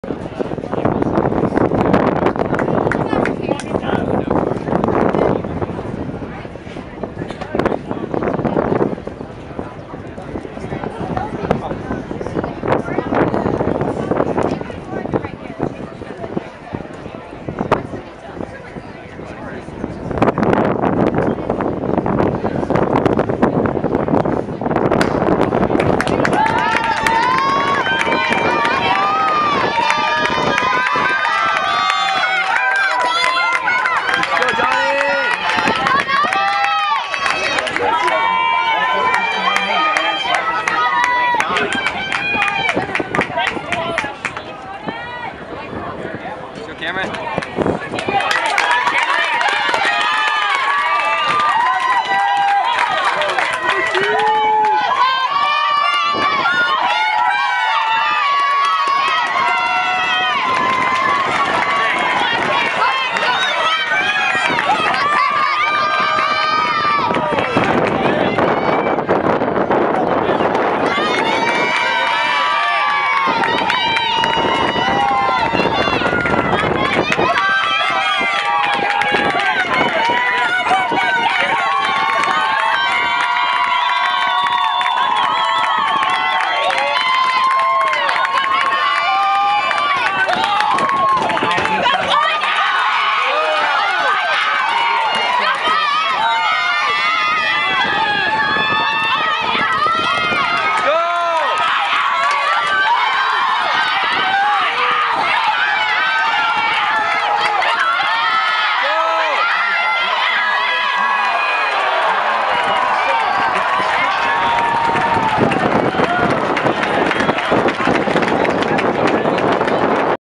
GRAPEVINE FAITH 4X200M RELAY STATE RUNNER-UP AT TAPPS 4A STATE TRACK & FIELD CHAMPIONSHIPS
KEYSHAWN WYATT, SETH TEXADA, PRESTON STEELE, JAKE MITCHELL CLOSE THE 2016 TRACK SEASON WITH A 2ND PLACE FINISH. CONGRATULATIONS ON A GREAT YEAR